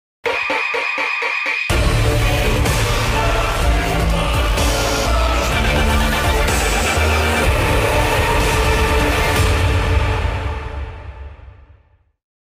Fan-Made Death Battle Trailer | Perfuma VS Poison Ivy (She-Ra VS DC Comics)
Nature is a such beautiful gift to the world. From the trees to the flowers blooming in the sun to the wind and the grass. However, mother nature can also be dangerous as it can be used as a weapon for fighting or destruction. These women have the power of nature at the beck of their call and once you get in a battle with them, let's hope you don't get buried six feet under.